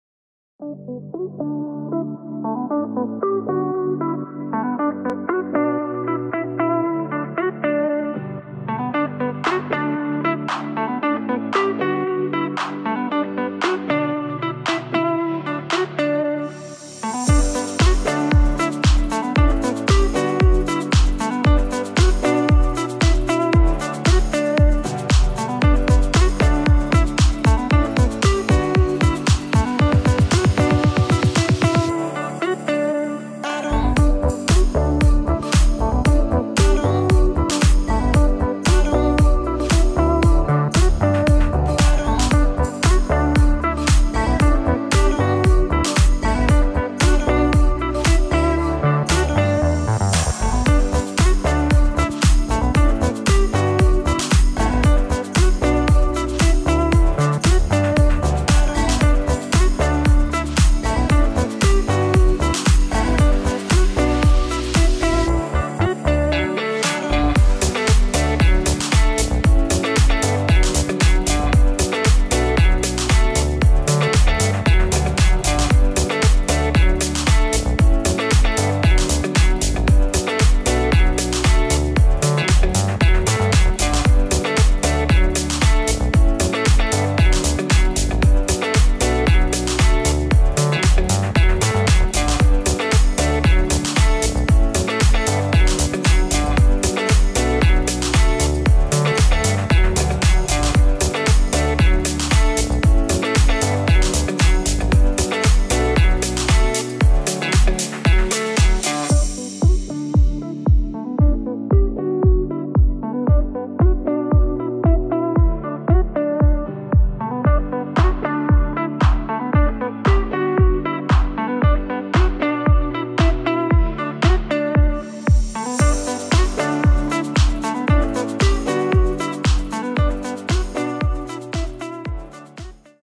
TE KOOP Leiderdorp appartement 4/5 kamers Kievitstraat 11
Modern en mooi 4/5 kamer appartement nabij park de Houtkamp en @Santhorst @winkelhof @vraagprijs € 389.000,- k.k.